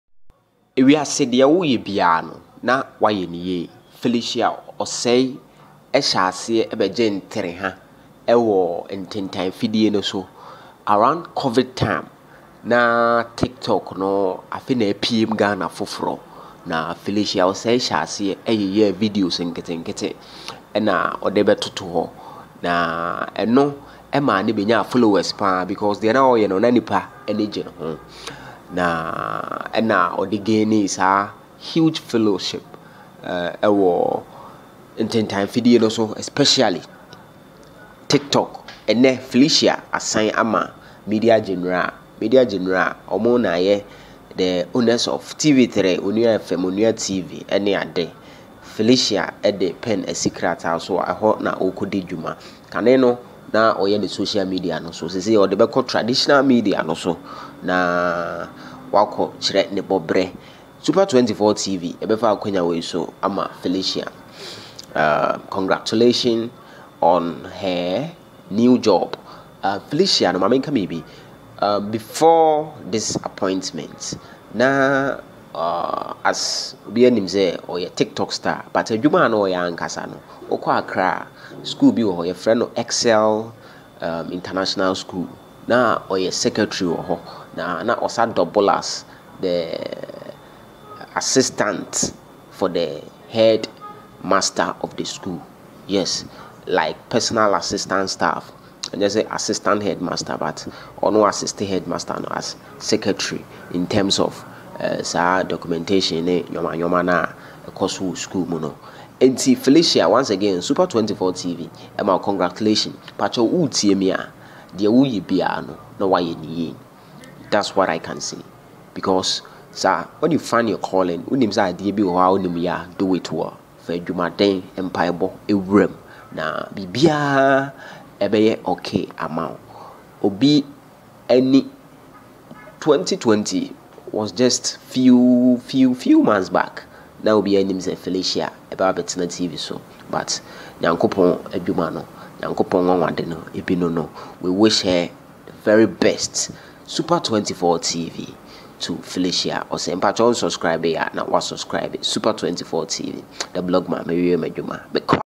TV3 Signs Tiktok Star Felicia Osei To Anchor A New TV/Radio Show On Onua TV and Onua FM
TV3 has grabbed Tiktok star Felicia Osei To anchor a show Onua TV and Onua FM